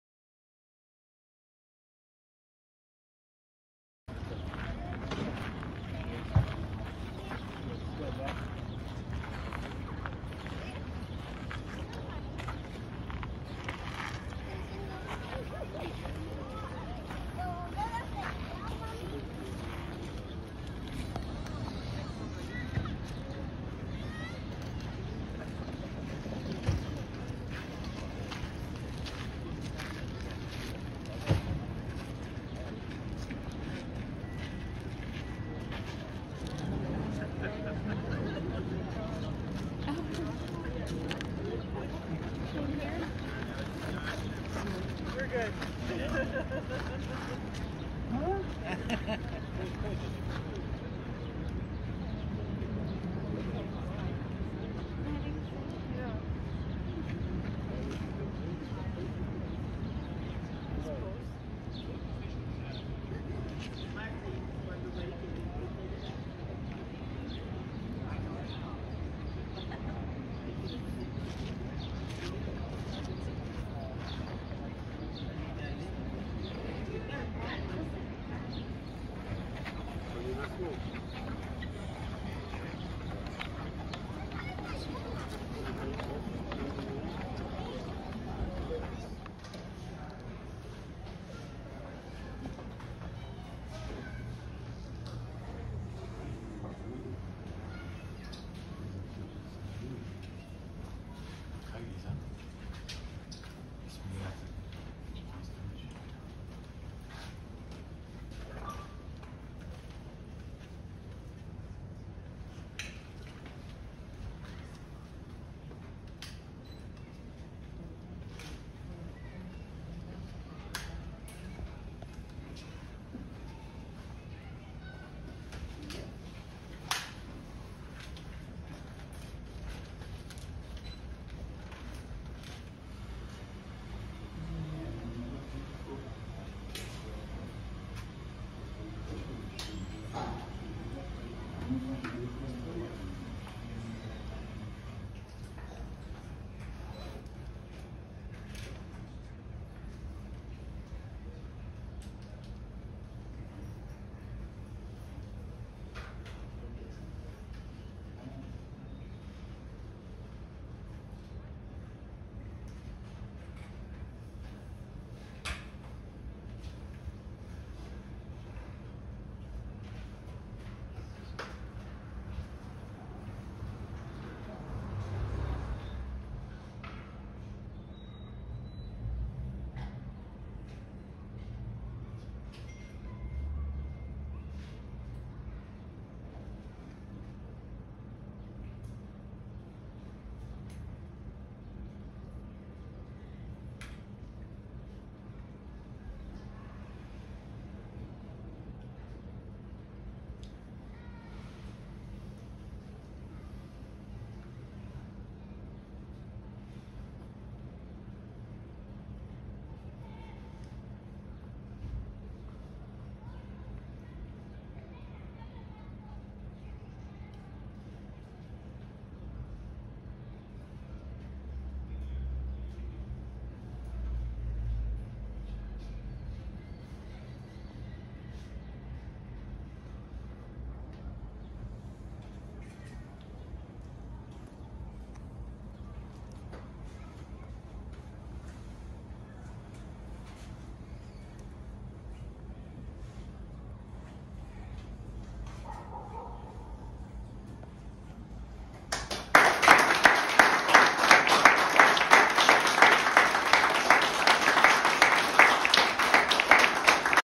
'Union' Performance by Neryth Yamile Manrique and Cecilia Stelini
Documentation of 'Union' Performance by Neryth Yamile Manrique and Cecilia Stelini for the Old Growth exhibition @ Propeller Art Gallery, August 15, 2022